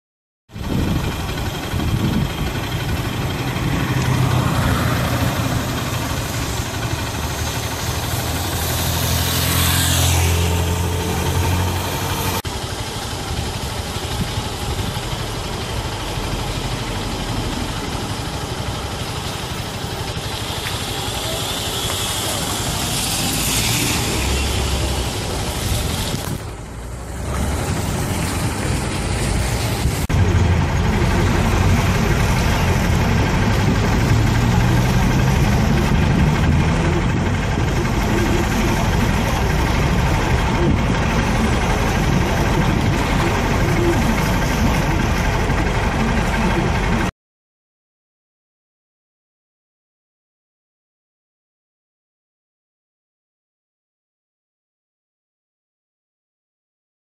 Buracos e esgotos atrapalham o trânsito na av. das Fronteiras
Buracos e esgotos atrapalham o trânsito em diversos pontos na  av. das Fronteiras , região norte da capital.